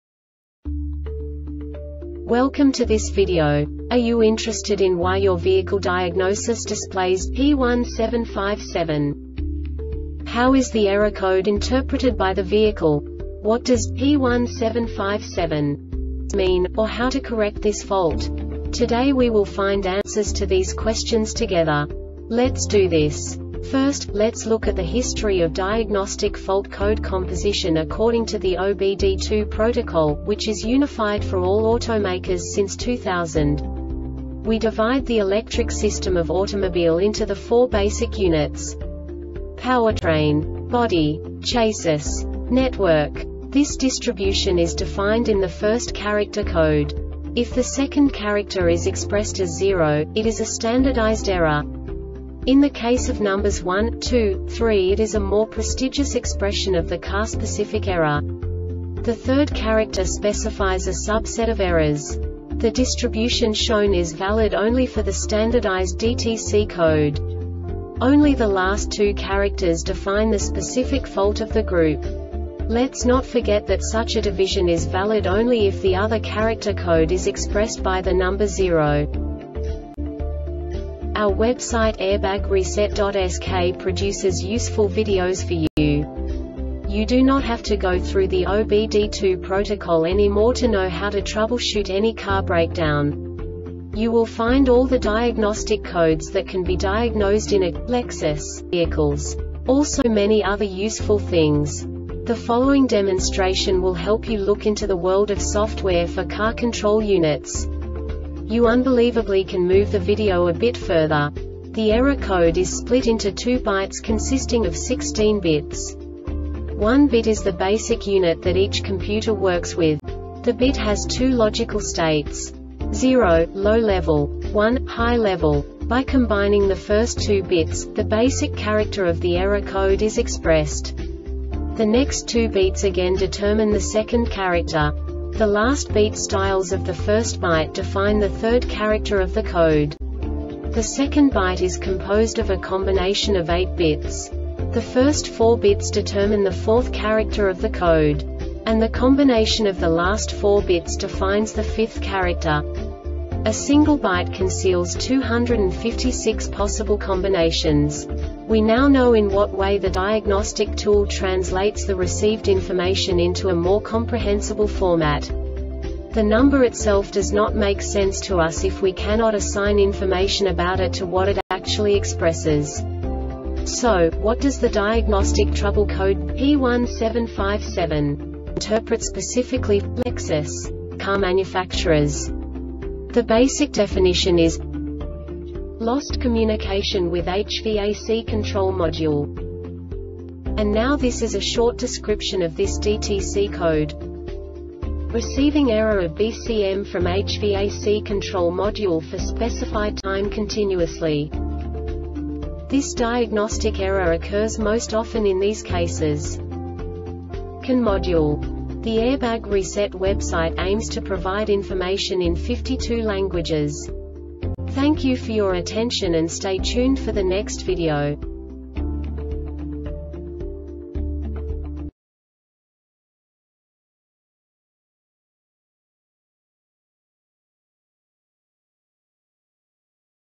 DTC Lexus P1757 Short Explanation
Contents:
0:21 Basic DTC analysis according to OBD2 protocol standard.
1:48 Insight into programming
2:58 A diagnostically understandable description of the Diagnostic Trouble Code
3:05 Basic error definition

Make:
Lexus
Code:
P1757
Definition:
Lost Communication With HVAC Control Module
Description:
Receiving error of BCM from HVAC control module for specified time continuously.
Cause:
CANModule